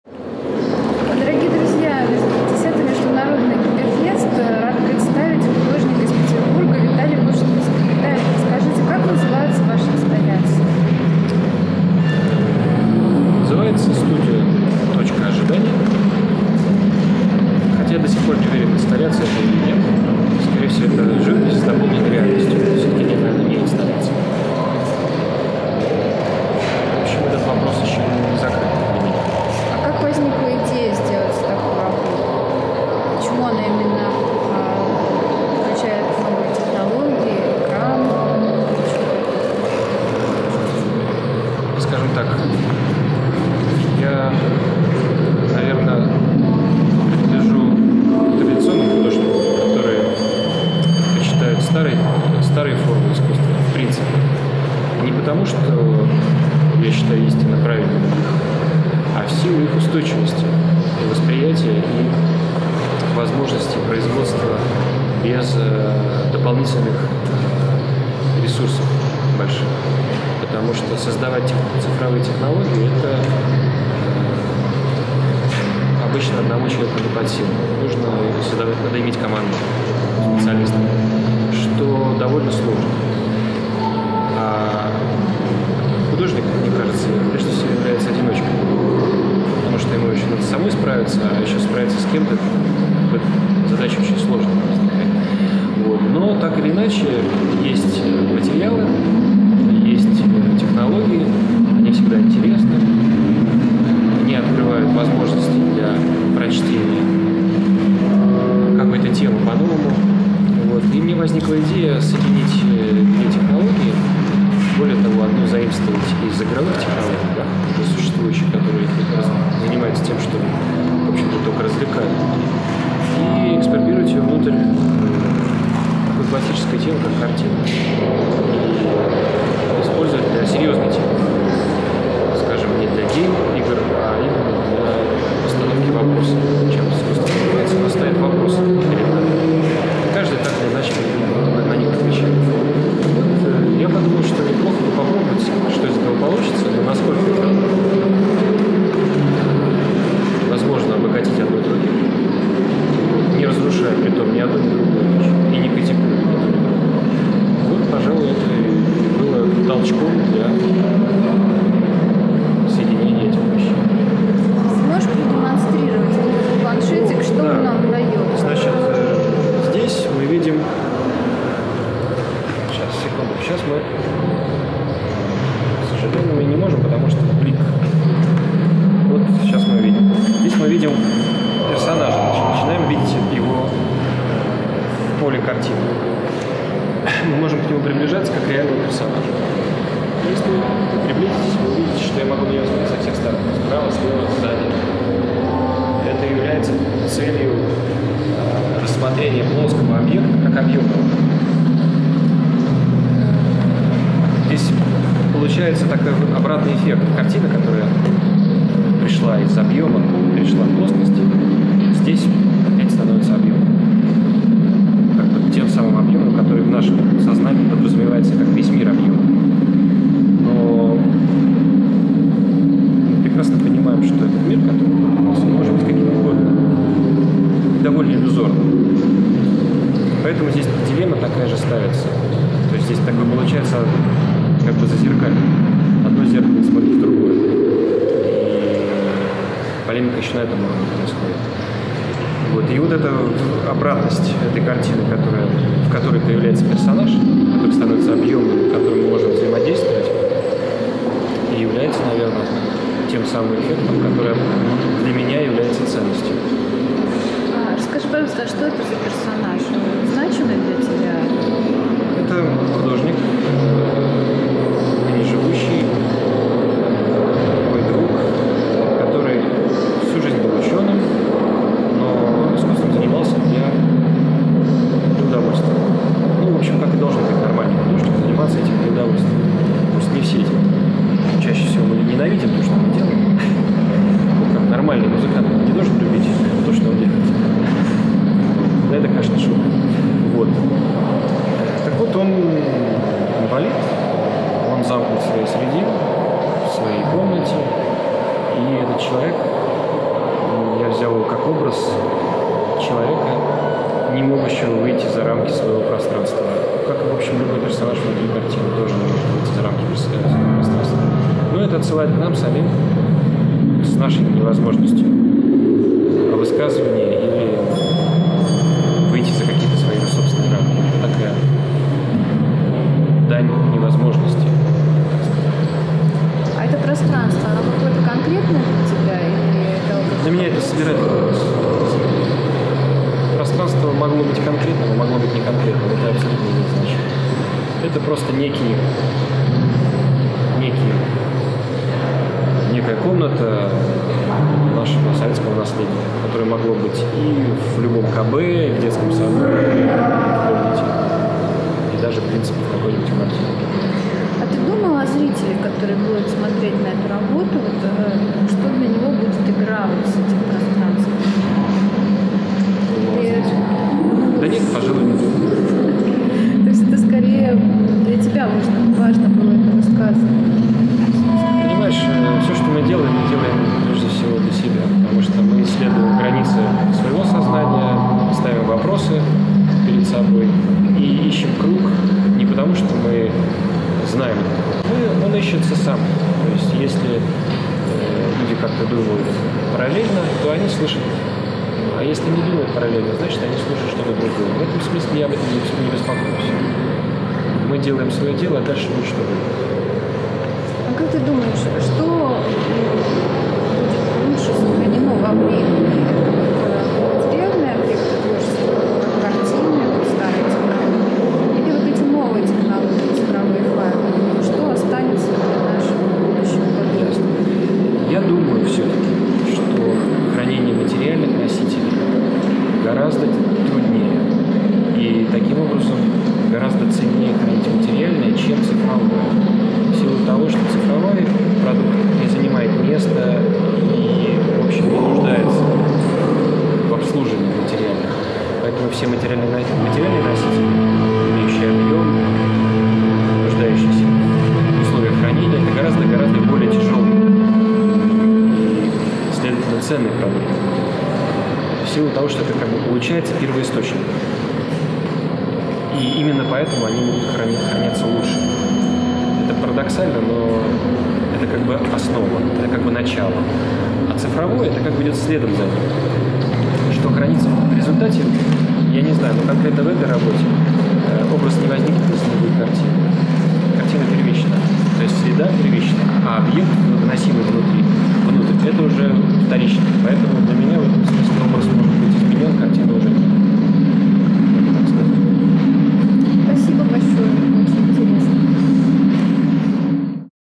CYFEST 10 - Интервью с Виталием Пушницким
Виталий Пушницкий

Художник. Родился в 1967 году в Ленинграде (СССР). Окончил Институт живописи, скульптуры и архитектуры им. И.Е.Репина (Ленинград, СССР). Участник крупных групповых выставок в Европе, США и России, в том числе Венецианской биеннале (параллельная программа, Италия, 2007) и Московской биеннале (Россия). Имел персональные выставки во многих музеях и галереях в России и по всему миру. Работы Пушницкого находятся в собраниях Русского музея (Санкт-Петербург, Россия), Московского музея современного искусства (Москва, Россия) и в частных коллекциях. Живет и работает в Санкт-Петербурге (Россия).

Vitaly Pushnitsky

Artist. Born in 1967 in Leningrad, USSR. Graduated from the Ilya Repin State Academy Institute of Painting, Sculpture and Architecture (Leningrad, USSR). Pushnitsky participated in major group exhibitions in Europe, USA and Russia, such as Venice Biennale (parallel program, Italy, 2007), Moscow Biennale (Russia), and had numerous solo shows in museums and galleries in Russia and around the world. His works are in various public collections, such as Russian Museum (St. Petersburg, Russia) and Moscow Museum of Modern Art (Russia), and in many private collections. Lives and works in St. Petersburg, Russia.